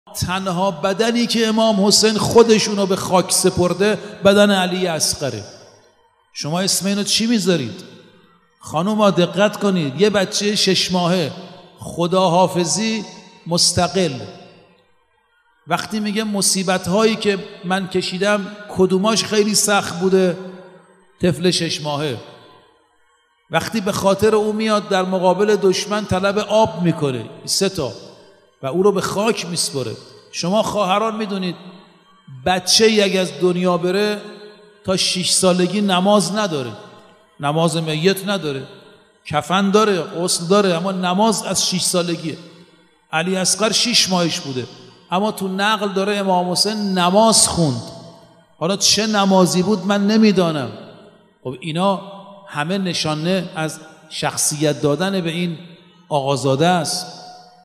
روضه حضرت علی اصغر علیه السلام در مراسم شیرخوارگان حسینی
روضه حجت الاسلام و المسلمین رفیعی  در وصف مقام حضرت علی اصغر علیه السلام در مراسم شیرخوارگان حسینی